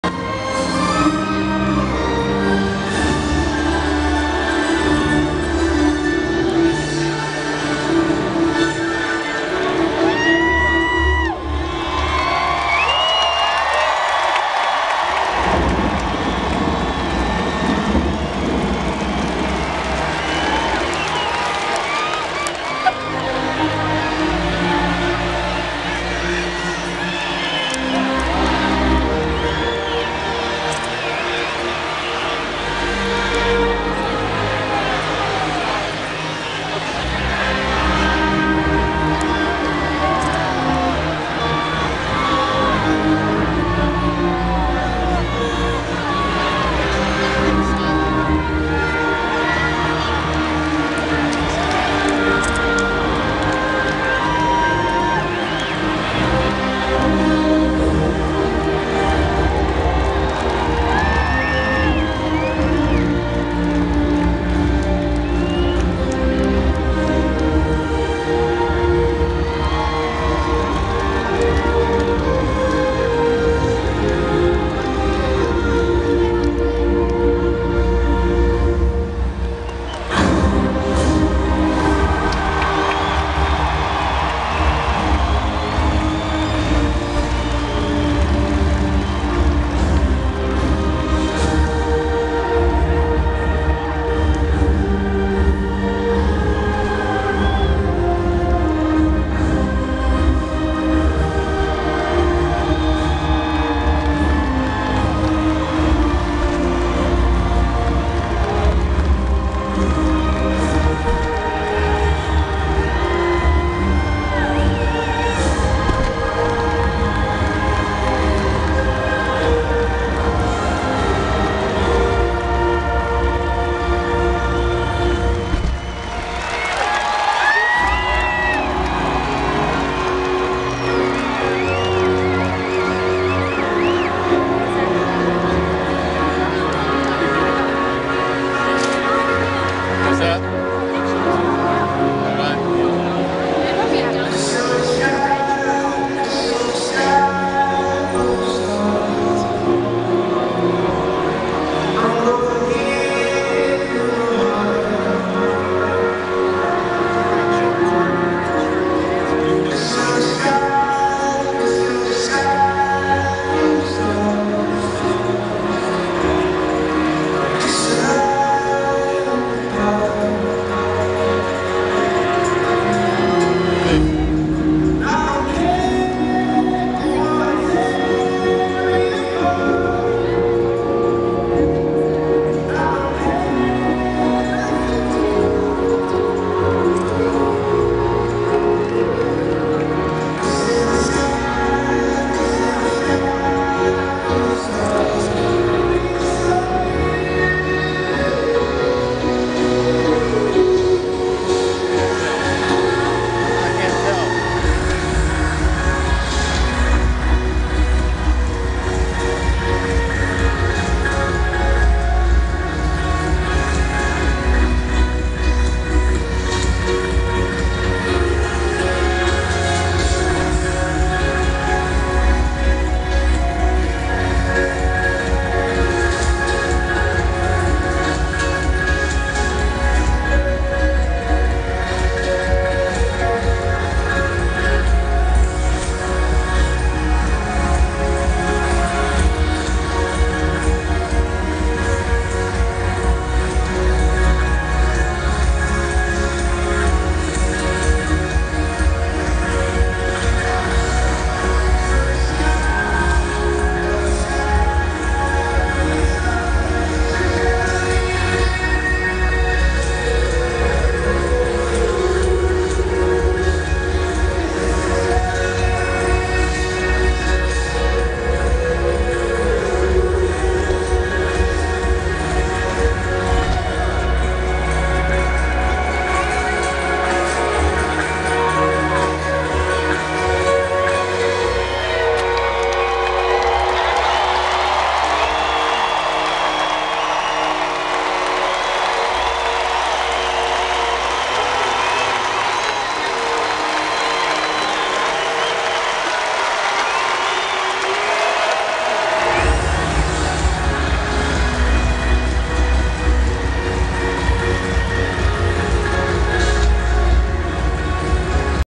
New scoreboard at Everbank Field
Unveiling of the new scoreboard at the home of the Jacksonville Jaguars